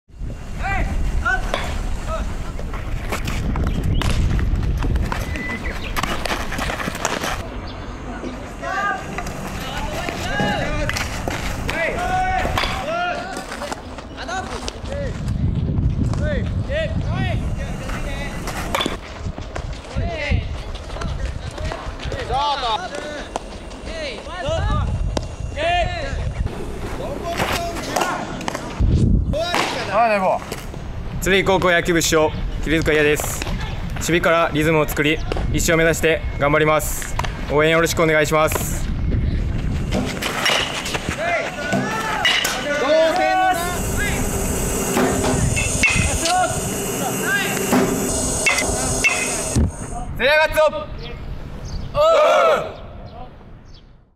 県高校野球大会頂点目指す30校・つるぎ高校
守備からリズムを作り、１勝を目指して頑張る。